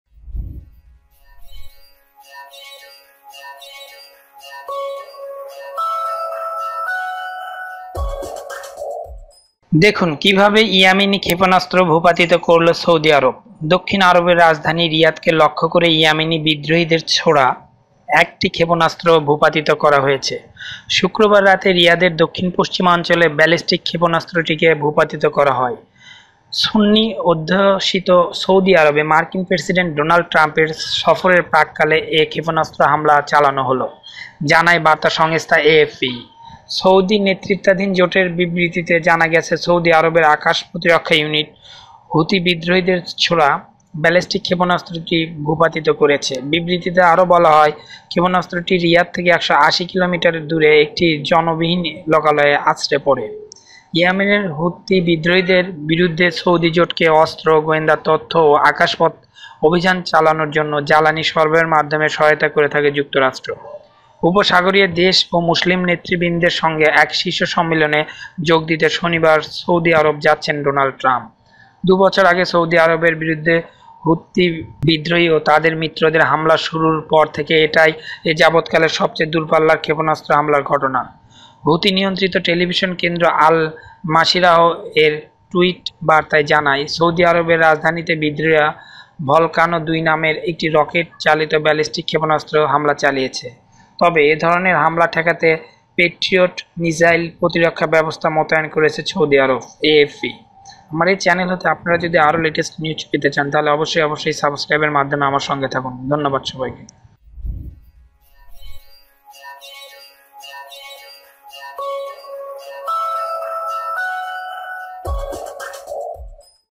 দেখুন কিভাবে ইয়েমেনী ক্ষেপণাস্ত্র ভূপাতিত করলো সৌদি আরব
সৌদি আরবের রাজধানী রিয়াদকে লক্ষ্য করে ইয়েমেনী বিদ্রোহীদের ছোঁড়া একটি ক্ষেপণাস্ত্র ভূপাতিত করা হয়েছে। শুক্রবার রাতে রিয়াদের দক্ষিণ-পশ্চিমাঞ্চলে ব্যালাস্টিক ক্ষেপণাস্ত্রটিকে ভূপাতিত করা হয়। সুন্নি অধ্যুষিত সৌদি আরবে মার্কিন প্রেসিডেন্ট ডোনাল্ড ট্রাম্পের সফরের প্রাক্কালে এ ক্ষেপণাস্ত্র হামলা চালানো হলো, জানায় বার্তা সংস্থা এএফপি।
সৌদি নেতৃত্বাধীন জোটের বিবৃতিতে জানা গেছে, সৌদি আরবের আকাশ প্রতিরক্ষা ইউনিট ‘হুতি বিদ্রোহীদের ছোঁড়া ব্যালাস্টিক ক্ষেপণাস্ত্রটি ভূপাতিত করেছে।’ বিবৃতিতে আরো বলা হয়, ক্ষেপণাস্ত্রটি রিয়াদ থেকে ১৮০ কিলোমিটার দূরের একটি জনবিহীন লোকালয়ে আছড়ে পড়ে।
ইয়েমেনের হুতি বিদ্রোহীদের বিরুদ্ধে সৌদি জোটকে অস্ত্র, গোয়েন্দা তথ্য ও আকাশ পথে অভিযান চালানোর জন্য জ্বালানি সরবরাহের মাধ্যমে সহায়তা করে থাকে যুক্তরাষ্ট্র। উপসাগরীয় দেশ ও মুসলিম নেতৃবৃন্দের সঙ্গে এক শীর্ষ সম্মেলনে যোগ দিতে শনিবার সৌদি আরব যাচ্ছেন ডোনাল্ড ট্রাম্প।
দুই বছর আগে সৌদি আরবের বিরুদ্ধে হুতি বিদ্রোহী ও তাদের মিত্রদের হামলা শুরুর পর থেকে এটাই এযাবতকালের সবচেয়ে দূরপাল্লার ক্ষেপণাস্ত্র হামলার ঘটনা। হুতি নিয়ন্ত্রিত টেলিভিশন কেন্দ্র আল-মাসিরাহ এর টুইটবার্তায় জানায়, ‘সৌদি আরবের রাজধানীতে বিদ্রোহীরা ভলকানো-২ নামের একটি রকেট চালিত ব্যালাস্টিক ক্ষেপণাস্ত্র হামলা চালিয়েছে।’
তবে এ ধরনের হামলা ঠেকাতে প্যাট্রিওট মিসাইল প্রতিরক্ষা ব্যবস্থা মোতায়েন করেছে সৌদি আরব। এএফপি।